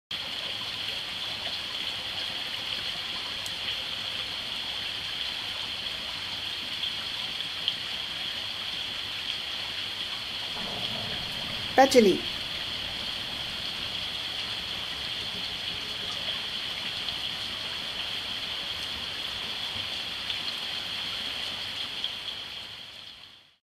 badjili
This animation comes from watching and listening to rain falling in our garden. I made this after learning some Bundjalung language from Drew Roberts when he was teaching at Eora TAFE NSW. I was told that 'badjili' is the sound of rain striking the ground.